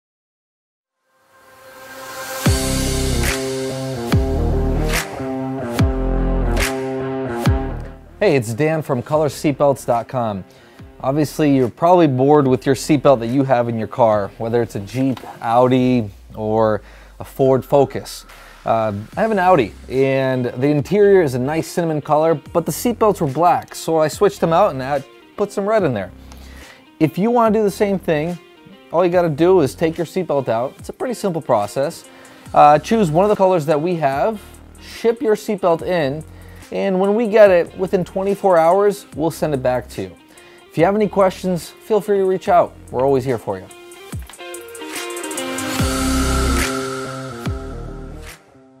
Get Custom Color Seat Belts - The Process
We re-stich your OEM tags and offer a 24hr turnaround and a lifetime warranty! Ready to get started?